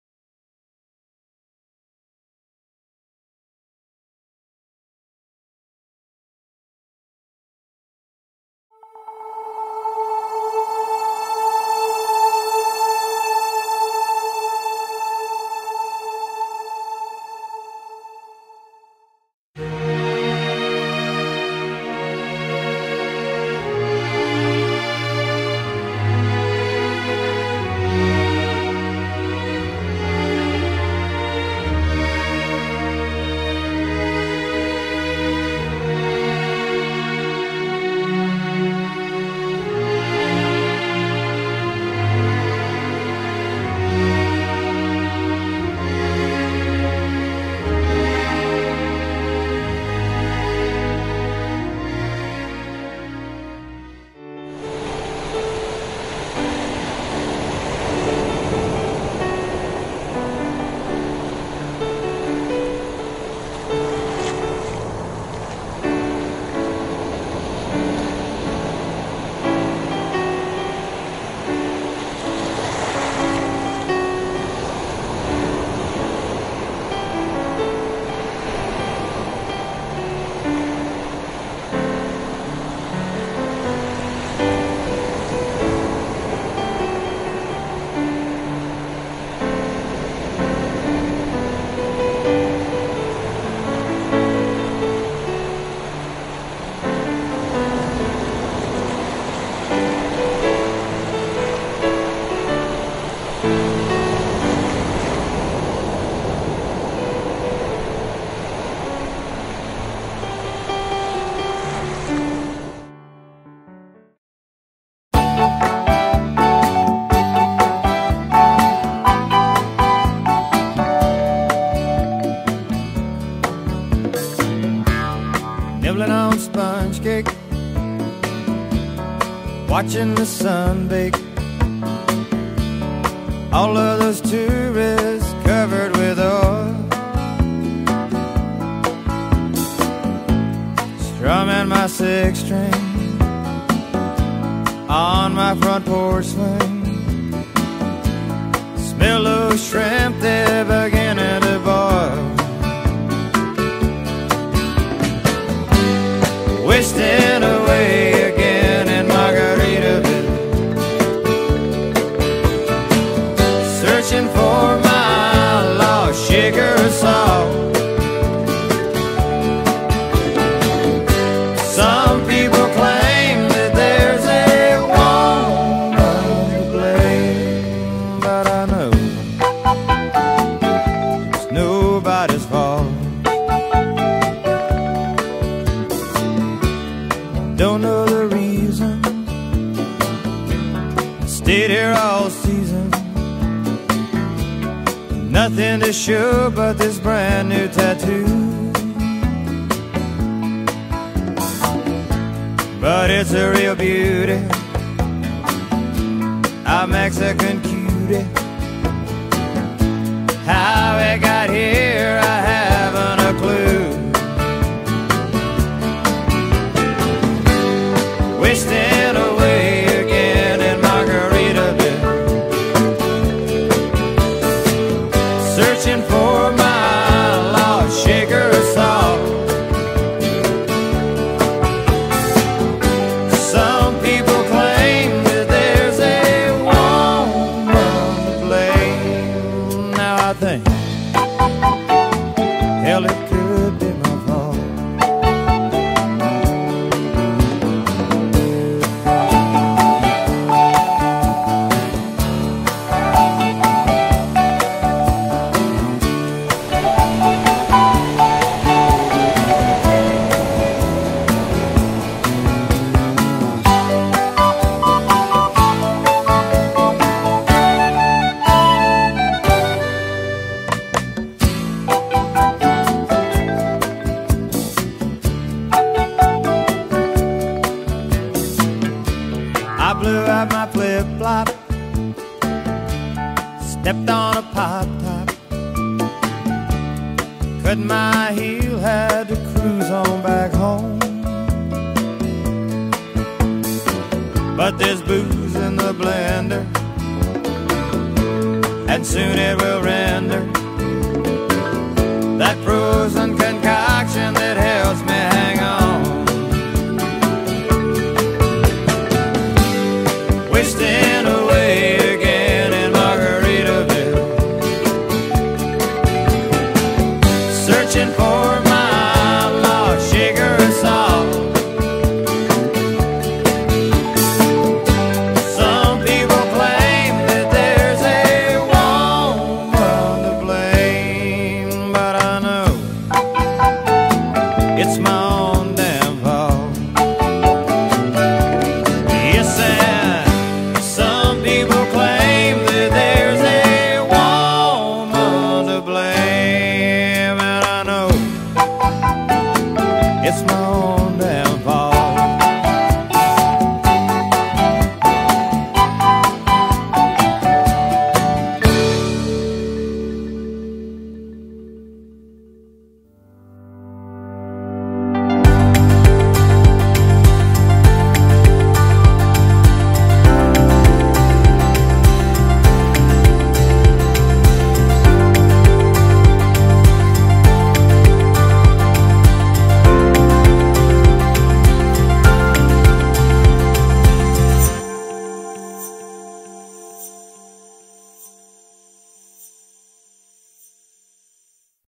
REMEMBERING JIMMY BUFFETT
Jimmy Buffett died at the age of 76. He has been described as "the tropical troubadour whose folksy tunes celebrated his laidback lifestyle."  In the words of Paul McCartney, " he lived his life like a song and enjoyed every minute of it up until he died."  Best viewed in 4k.